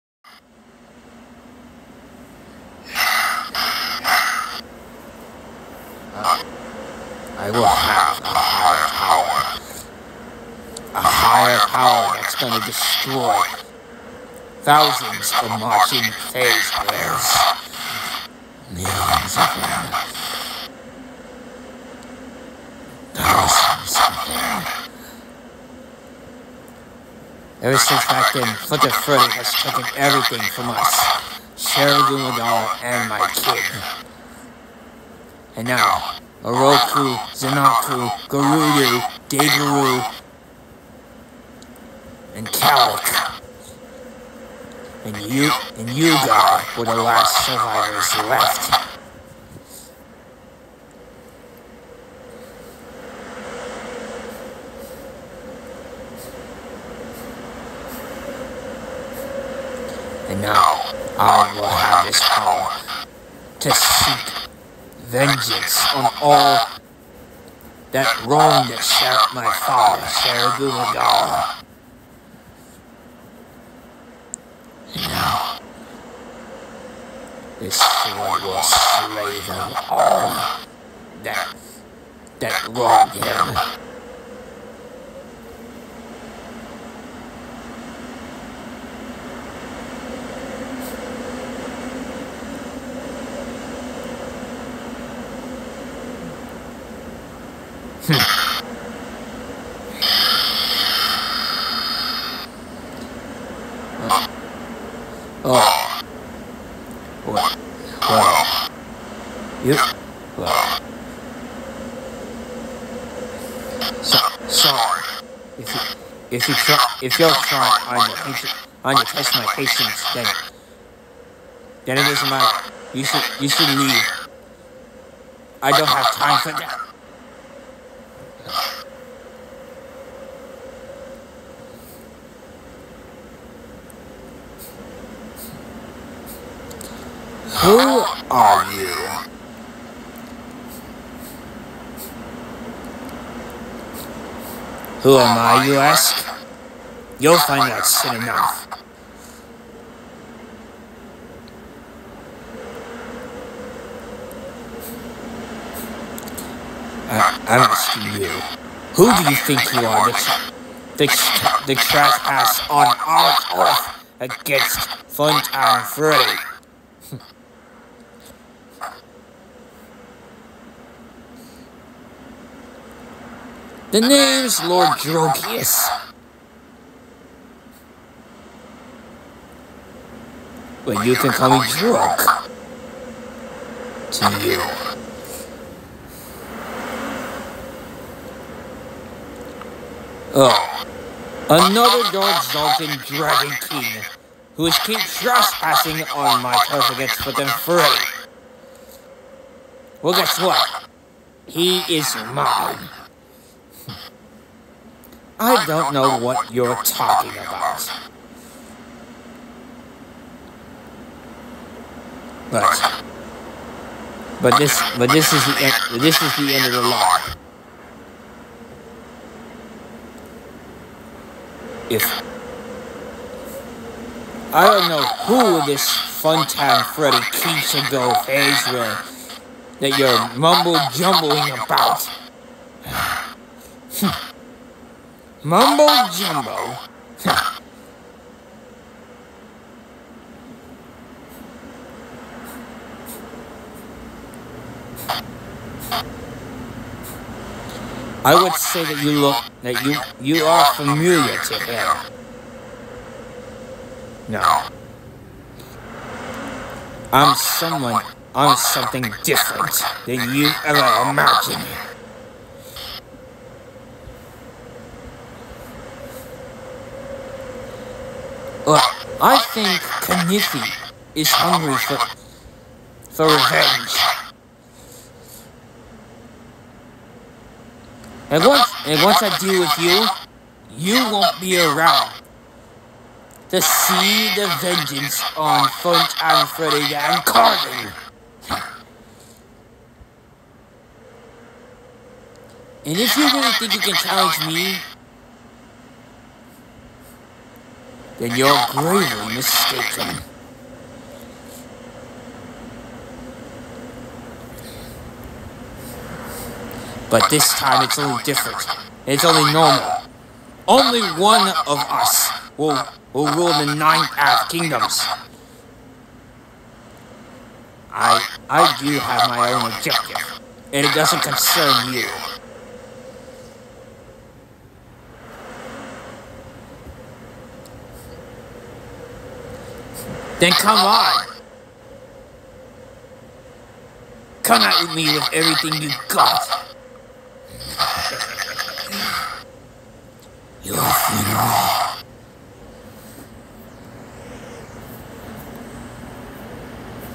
Lord Drokius Vs Garodu Magala: The Ferocious Madness Brawl of The Dark Xaldjinn Dragons
Garodu Has Been Beaten By Funtime Freddy That’s Me. He Has Healing For 30 Hours. He’s Been Itching For A Rematch Since He Beat Him.
But Suddenly A Powerful Mysterious Dark Xaldjinn Dragon King Shows Up out of Nowhere
And Questions Garodu Magala About his Objective. But Garodu Says That Doesn’t Concern Him. So He Decides To Tell him Who Is He. The Mysterious Figure said He Will Find Out soon Enough. So Garodu Decides To Brawl him in a Battle. The Two Engage Conflict Between The Two Dark Xaldjinn Antagonistic Dragon Kings.